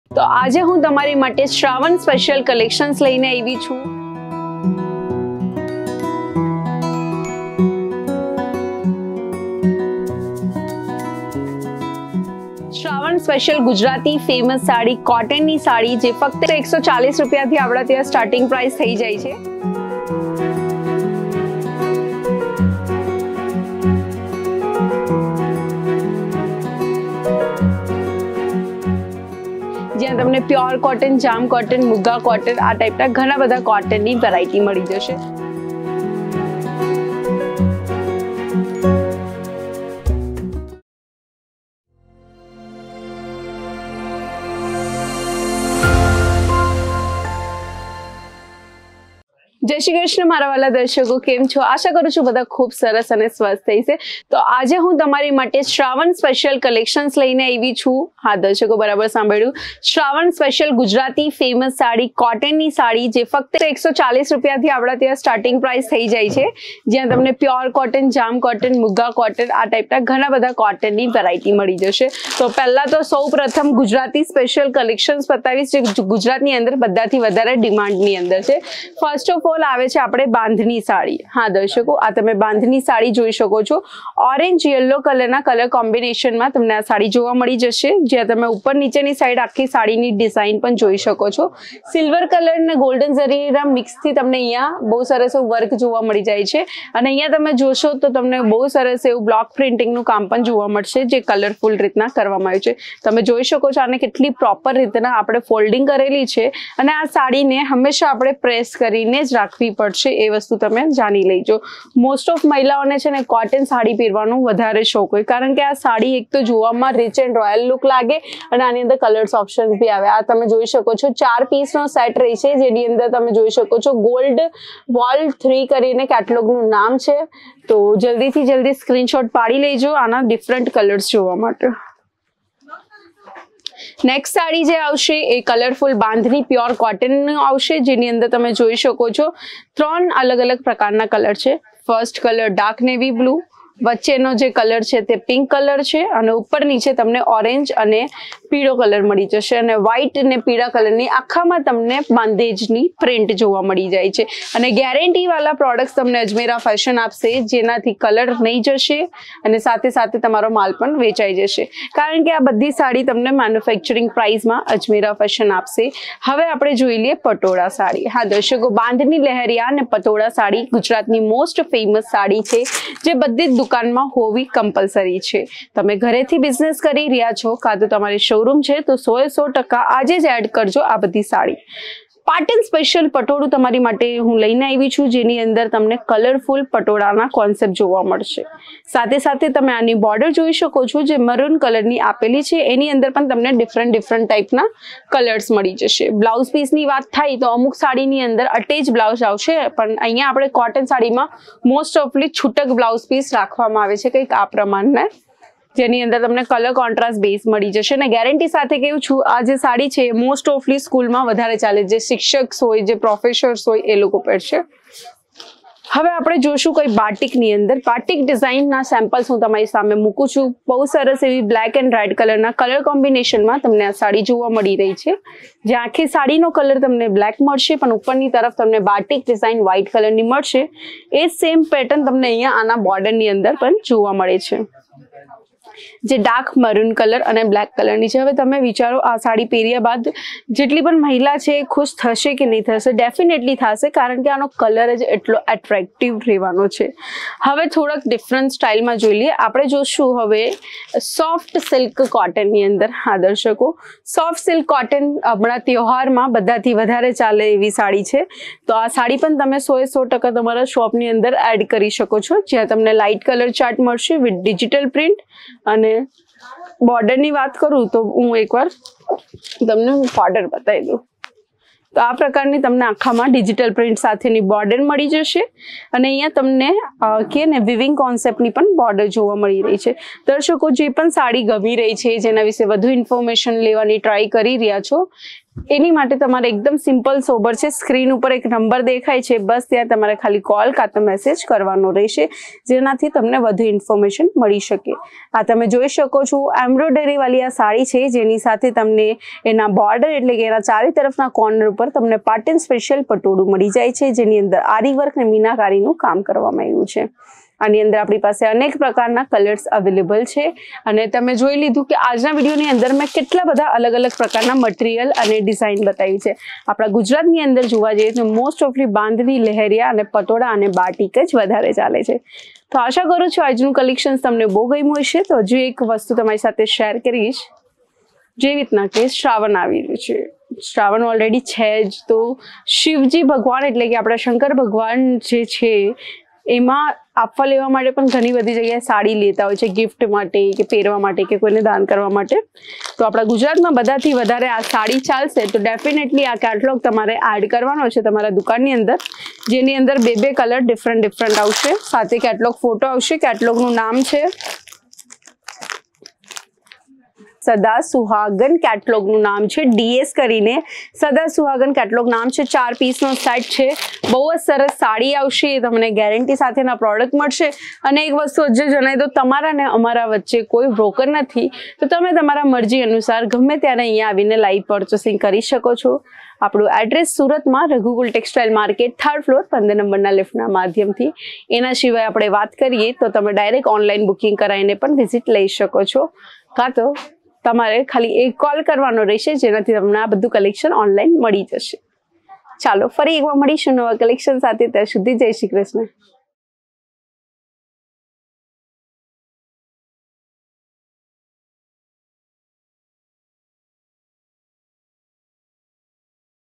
શ્રાવણ સ્પેશ્યલ ગુજરાત ની famous કોટન સાડી | Soft Silk Cotton Saree | Bandhani Cotton Saree
🎊Ajmera Fashion.

જય શ્રી કૃષ્ણ મિત્રો ,

આજ ના વિડિઓ માં સ્પેશ્યલ કલેકશન લઈને આવ્યા છે શ્રાવણ સ્પેશ્યલ ગુજરાત ની ફામૉસ કોટન સાડી ફક્ત 140/- થી શરુ થઇ જાય છે. જ્યાં તમને કોટન  , જામ કોટન , માગુ કોટન આ પ્રકાર ની વેરાયટી  મળી જશે

આ સાડીઓ ગુજરાત ની ફેમસ સાડીઓ છે .આ કલેકશન  તમારી દુકાન માટે એકદમ પરફેક્ટ છે કારણ કે આ સાડી નો ની ડિમાન્ડ અત્યારે  બહુ જ છે. જો તમારે ઘરે થી ધંધો કરી રહ્યા છો કે તમારો Show room  હોય તો 100 એ 100 % આ  કલેકશન Add કરો .

વધુ સાડીઓ જોવો હોય તો આ વીડિઓ જરૂર જોવો

તમારો આભાર
ટીમ અજમેરા ફેશન
સુરત